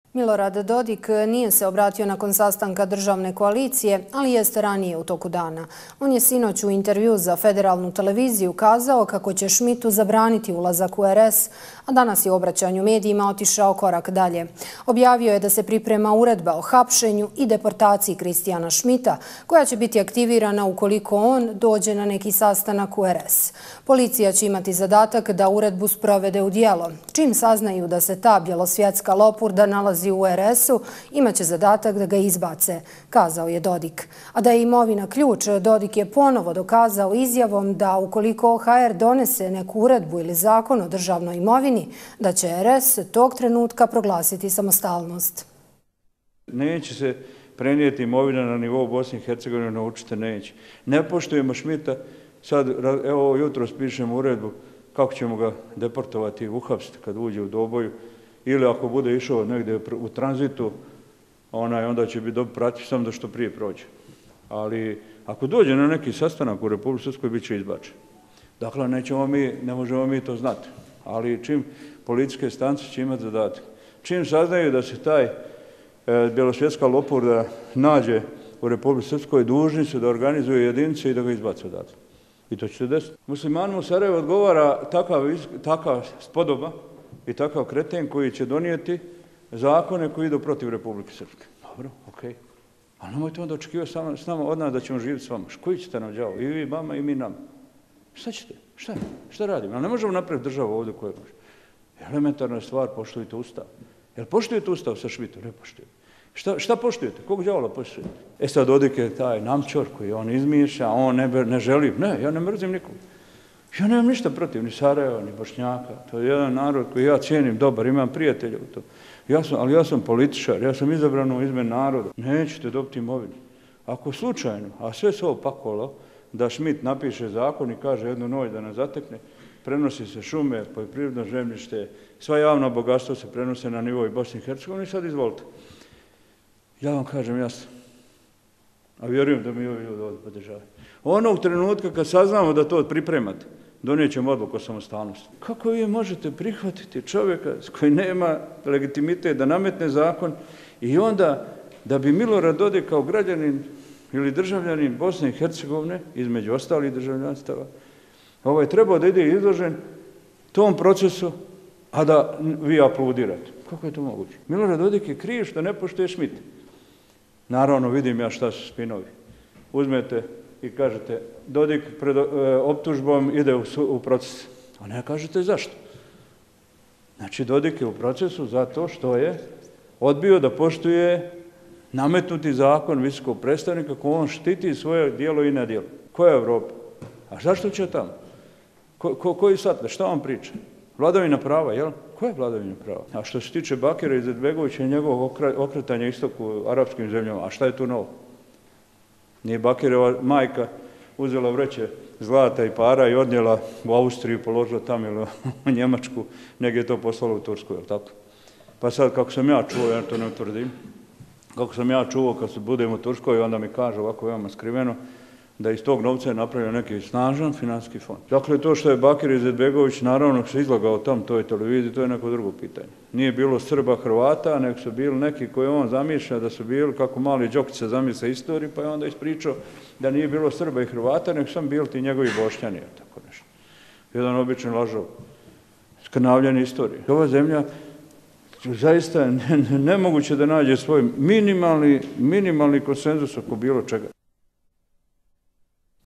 Dodik zaprijetio Šmitu: Ako dođe u RS uhapsit ćemo ga! Pripremamo uredbu o hapšenju i deportaciji!
Marketing, YouTube & Digital distribution:
EMDC Network